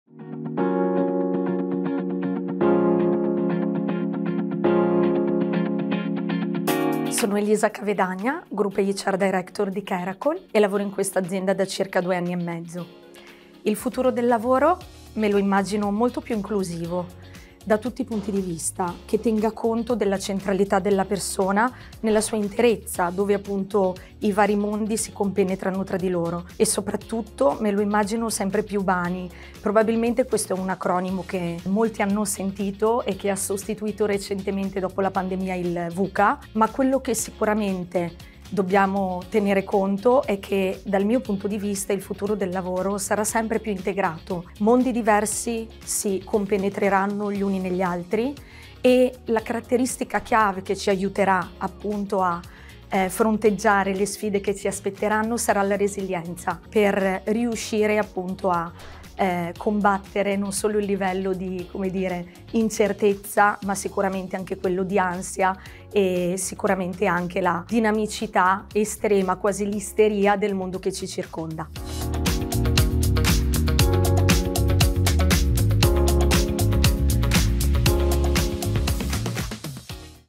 Agenda 2030 | Intervista a Elisa Cavedagna - Kerakoll
Grande successo di pubblico per la sesta edizione di AGENDA 2030, il più importante evento annuale promosso da WI LEGAL. Oltre 200 top manager di aziende italiane e internazionali si sono riuniti per condividere best practice, idee, riflessioni sui temi cruciali che stanno investendo il mondo del lavoro, un settore in continua evoluzione. 11 speaker d’eccezione si sono alternati sul palco con l’obiettivo di migliorare sempre di più i luoghi di lavoro, garantendo pari opportunità e qualità della vita alle persone, attraverso una gestione attenta delle risorse umane.

Guarda l'intervista a Elisa Cavedagna, Group HR Director di Kerakoll.